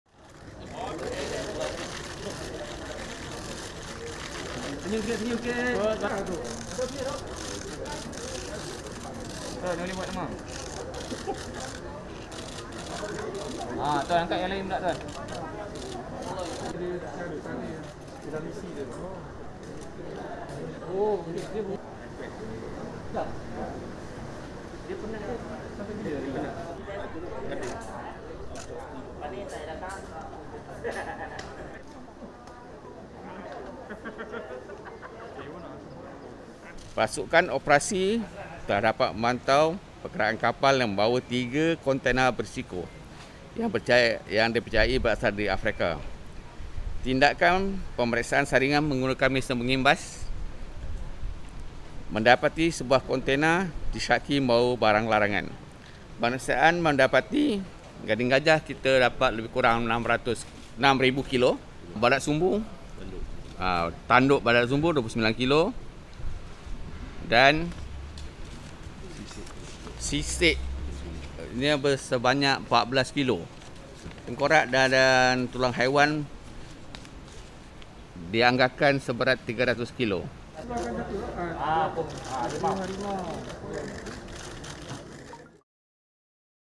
Malaysia seizes animal parts worth $18 mn | AFP
Malaysian customs officials say they have seized a stash of rare animal parts worth $18 million thought to have come from Africa, including elephant tusks, rhino horns and pangolin scales.